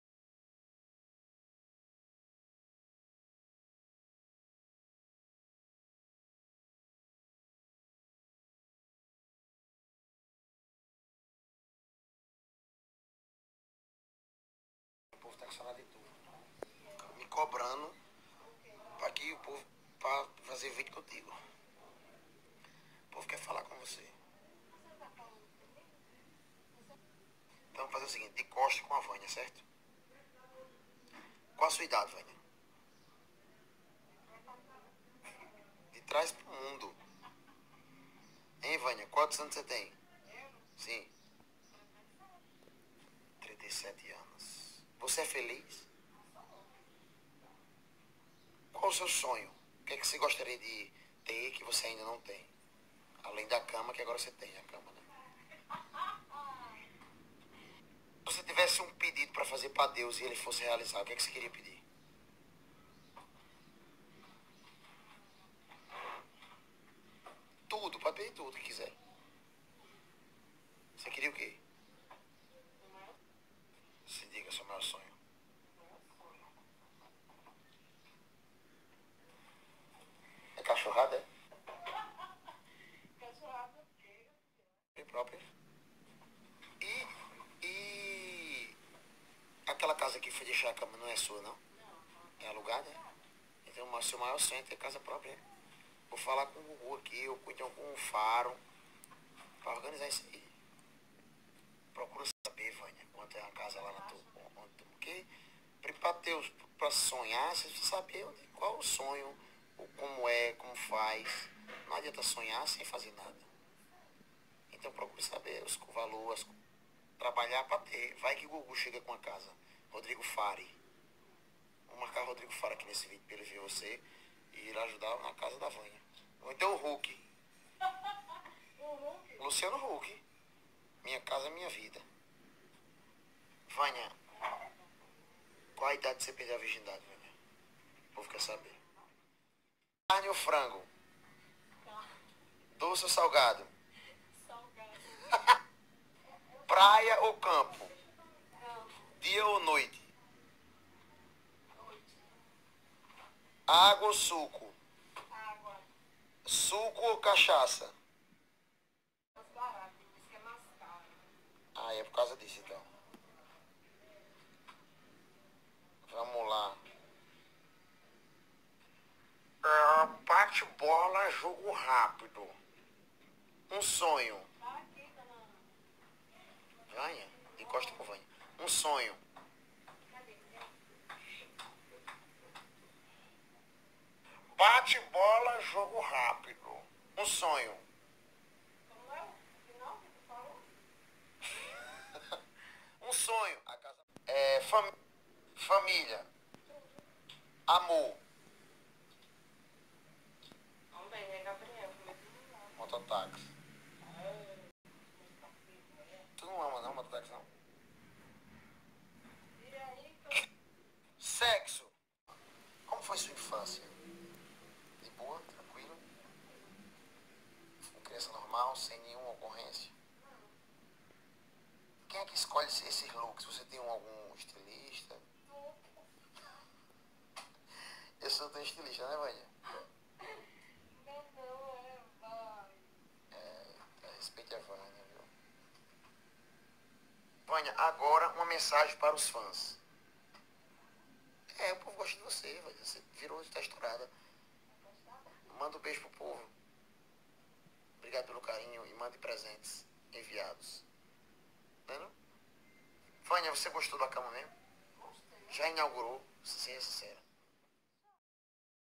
O grande sonho do GD era ver Vânia ganhar sua casa🏠 própria, vejam o vídeo e curtam p ajudar 🙏
Vânia tem um grande sonho em ganhar uma casa própria pois a dificuldade de pagar aluguel é grande,   o salario de doméstica é pouco pois ela tem outras contas à pagar, GD era testemunho de tudo isso,  vamos compartilhar esse vídeo pra chegar nas mãos de um apresentador famoso

Curtam e se inscrevam para  ajudar esse vídeo crescer